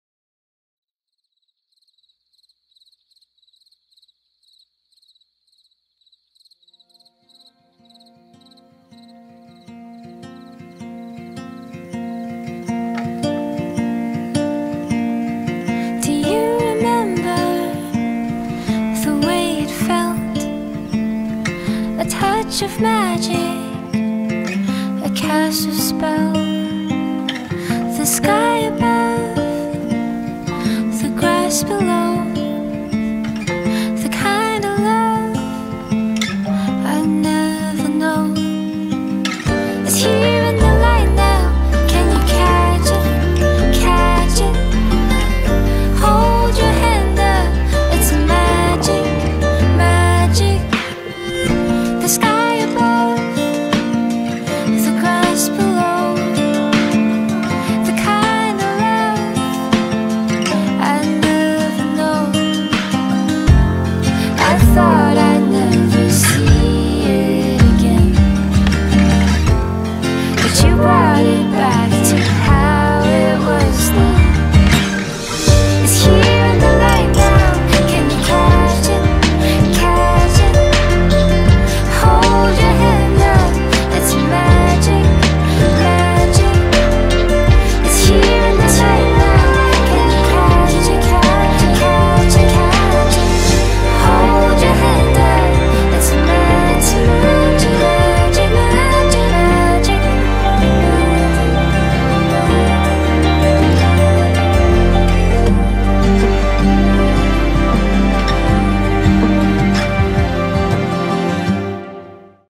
LUMI HANOI | CATCH THE LIGHT, FIND THE MAGIC | THẮP ÁNH SÁNG CHO TƯƠNG LAI RẠNG NGỜI
[English caption below]
✨ LUMI HANOI - THẮP ÁNH SÁNG CHO TƯƠNG LAI RẠNG NGỜI ✨

"🎶 Khi ánh sáng tỏa tràn
Ta say sưa đón lấy
Dang vòng tay nâng niu
Thấy diệu kỳ trước mắt…🎶"

Lần cuối cùng bạn khẽ vươn mình đón những tia nắng trong trẻo chảy dọc qua kẽ tay là khi nào?

🌤️ Hiện diện trên khắp nẻo đường, nhuộm vàng từng góc phố, song sự diệu kỳ của ánh sáng lại là điều chúng ta dễ dàng bỏ lỡ trong nhịp sống hối hả thường nhật.

🌱 Tách mình khỏi tất bật ồn ã, cùng tìm về Lumi Hanoi - chốn tia nắng vỗ về xúc cảm bình yên, nơi mọi bộn bề được hong khô bởi ánh bình minh trong trẻo bên hiên nhà.

💫 Khi mọi trải nghiệm lấp lánh dưới ánh dương cũng là lúc niềm vui kết nối thêm rạng ngời. Thổi bừng chất sống năng động nhiệt huyết để đam mê sáng tạo tỏa lan, Lumi Hanoi là nơi cảm thức thuộc về vẹn nguyên trong từng khoảnh khắc.
✨ LUMI HANOI - CATCH THE LIGHT, FIND THE MAGIC ✨

"🎶It's here in the light now
Can you catch it
Hold your hand up
It's magic, magic...🎶"

When was the last time you gently stretched out to welcome the crisp rays of sunlight streaming through your fingertips?

🌤️ Present on every street corner, painting each corner with a golden hue, yet the miraculous essence of light is often overlooked amidst the hustle and bustle of daily life.

🌱 Detach yourself from the hustle and bustle, and seek tranquility in Lumi Hanoi - where the gentle rays of sunlight caress your emotions, and where all worries are dried up by the clear dawn on the doorstep.
💫 As every experience sparkles under the sun, it's also the moment when joy connects and shines brighter. Ignite the radiant and passionate zest for life to let creativity flourish; Lumi Hanoi is where the sense of belonging remains intact in every moment.
Lumi Hanoi
Địa chỉ: Tây Mỗ, Nam Từ Liêm, Hà Nội